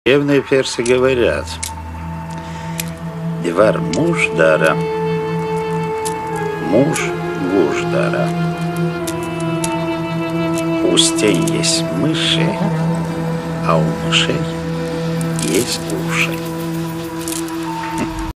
Məlik Dadaşov - Qədim Farslar deyirlər
Məlik Dadaşovdan Fars atalar sözü - Qədim Farslar deyirlər : "divarların içində siçanlar var, siçanların isə qulaqları var"!

Əziz Dostlar!